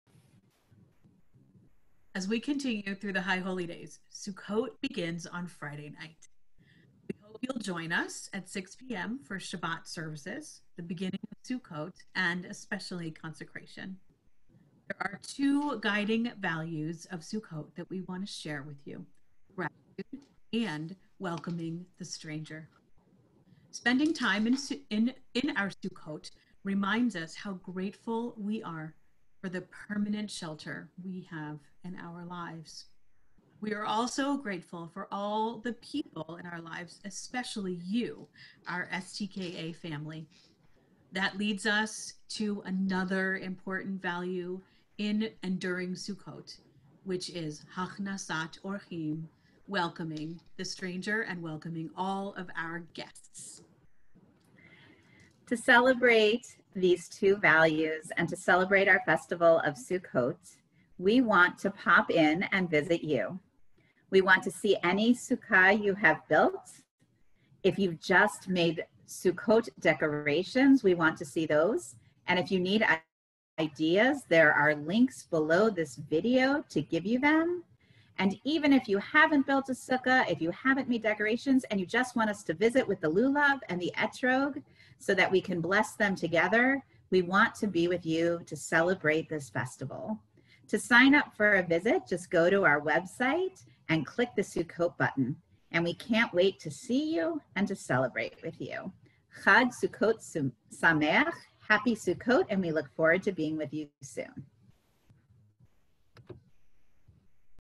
The Rabbis' VIDEO Torah Talk: October 1, 2020
Hungry? Build an edible sukkah!

Do you love to build? Have lots of legos or other building toys? Build a mini Lego sukkah!

Using leaves to decorate for Sukkot! What words can you think of to write on the leaves? What beautiful patterns do you see on the leaves that you can trace? All you need for this ageless project are leaves and indelible markers!